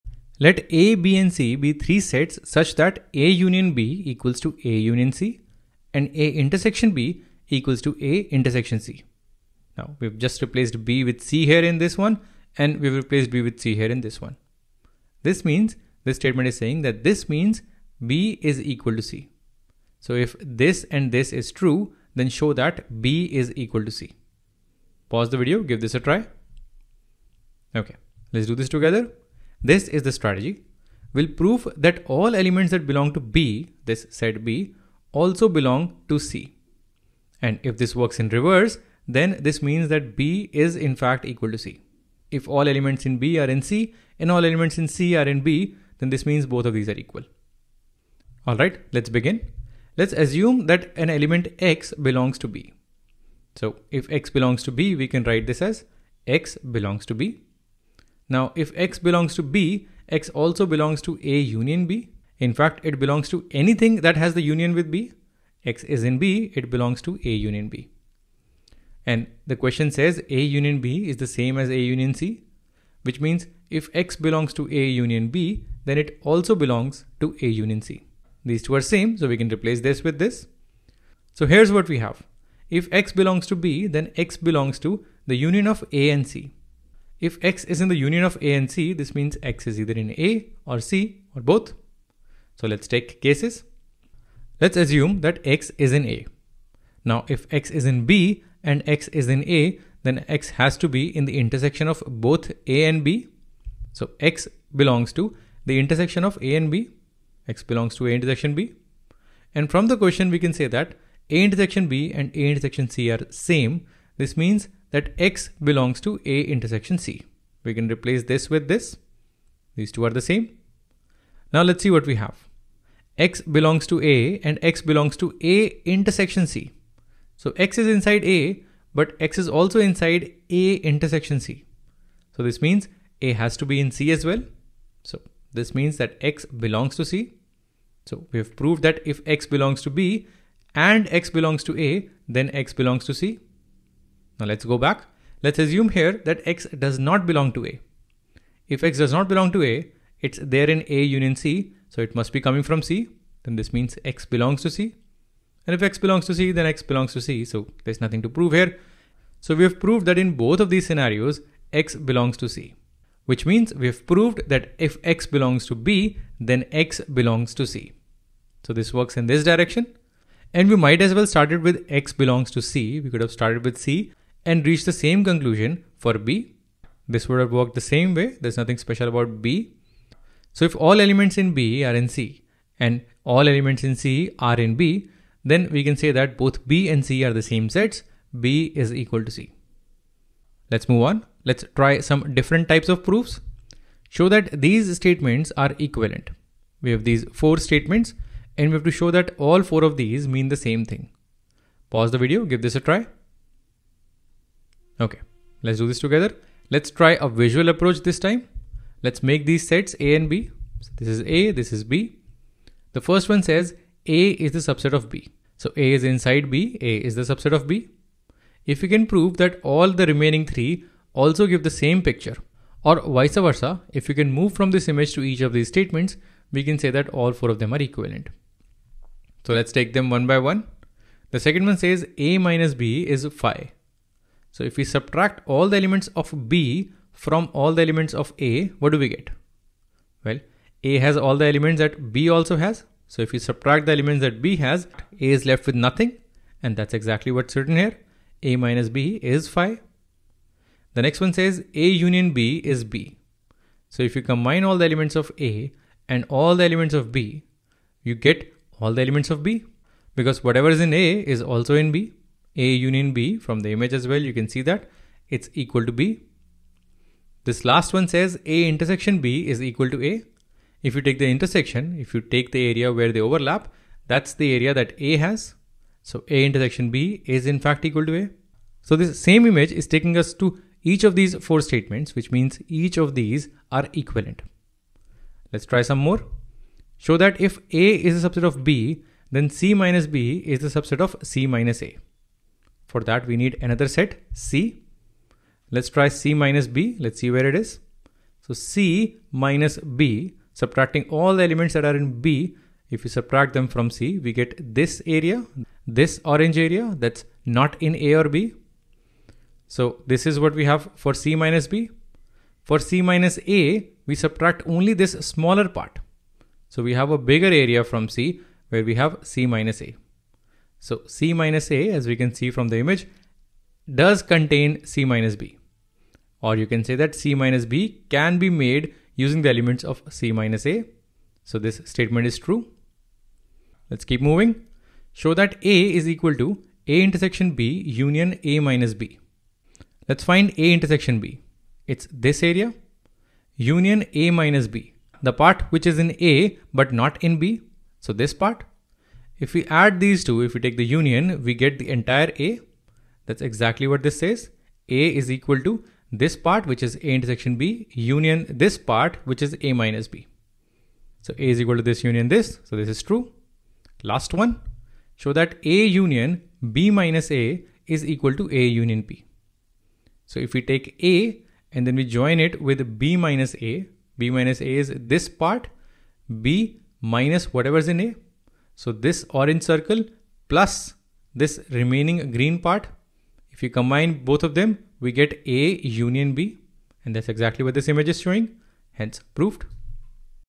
Applying properties of intersection and union of sets | Grade 11 | Math | Khan Academy
In this video, we apply properties of intersection and union of sets to prove given statements about sets. The first problem asks us to prove that two given sets are equal if they satisfy a given criteria. Next problem involves proving that four given statements are equivalent. The next set of problems involve combination of difference, intersection, and union of sets.

Courses on Khan Academy are always 100% free. Start practicing—and saving your progress—now!

Timestamps:
0:00 Show that B = C
3:10 Show that given statements are equivalent
4:45 Show that C - B is a subset of C - A
5:35 Another way to write the set A
6:05 Another way to write A union B

Created by
Ashish Gupta